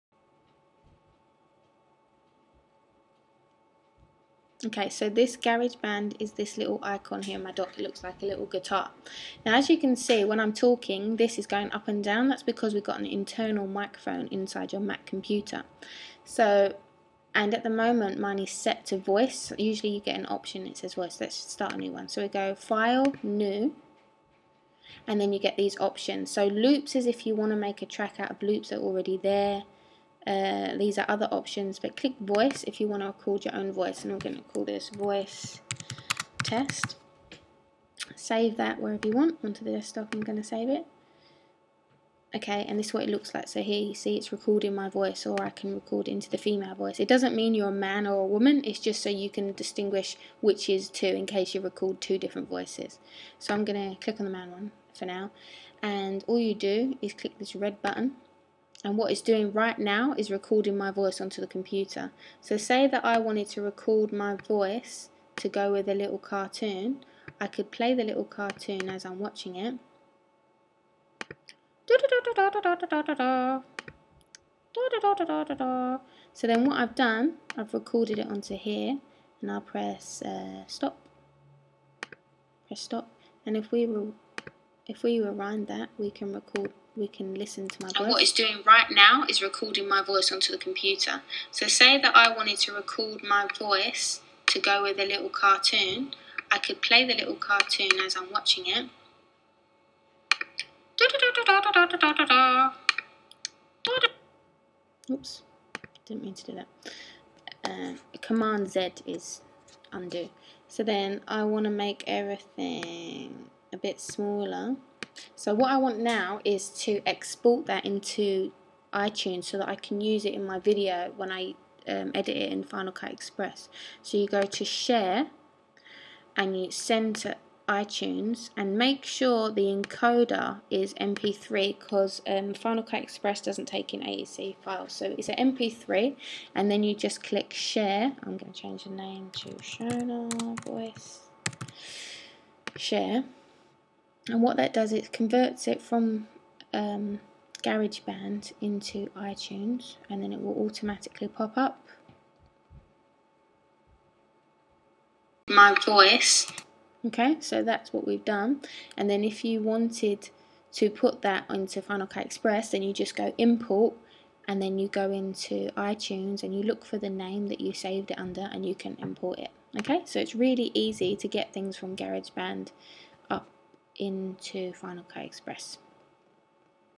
How to use garage band voice recording basics (Computer).m4v
Step by step guide to recording your voice into garage band and how to export to iTunes